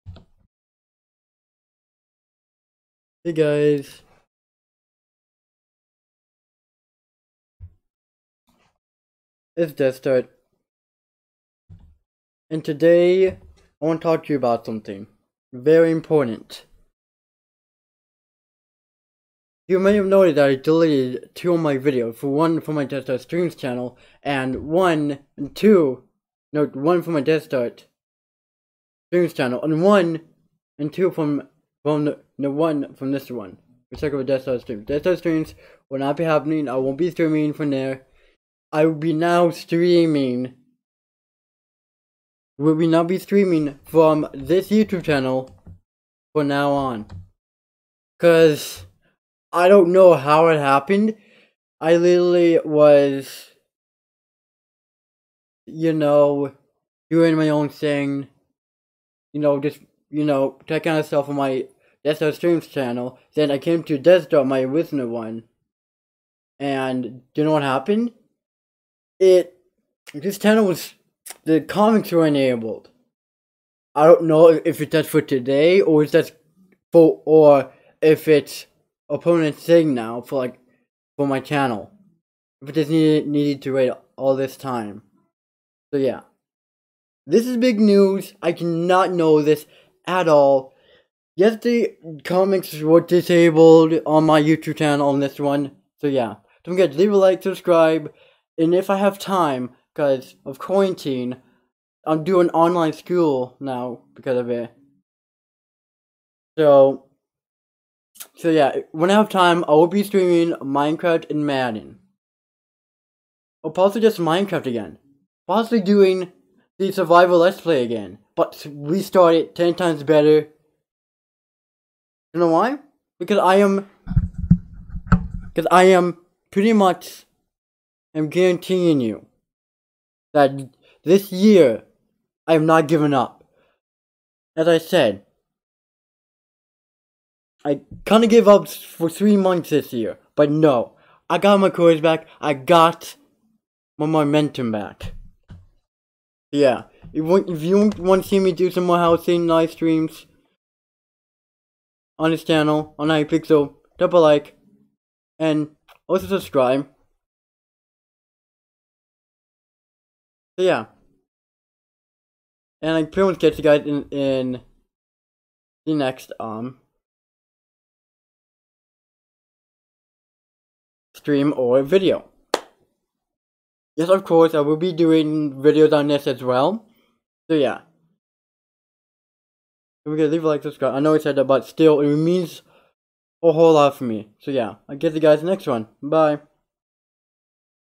I am back to streaming on this channel!!
That means that Deathdartstreams is no more and will never ever happen. well at-least for now. My comments are now back and that is why Deathdartstreams is no more. Lets hope for a better and wonderful Year for our channel.